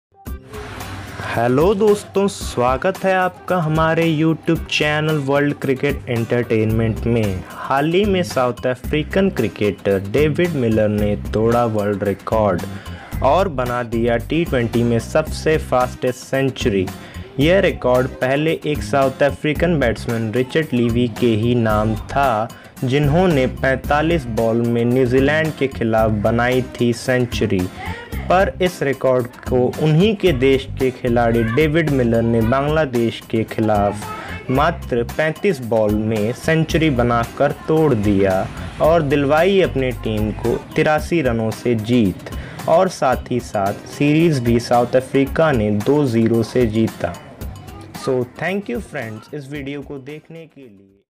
35 Ball में South African Batsman David Miller ने मारा Century - World Record - Cricket News
LAtest cricket news 2017 . most famous moments of indian cricket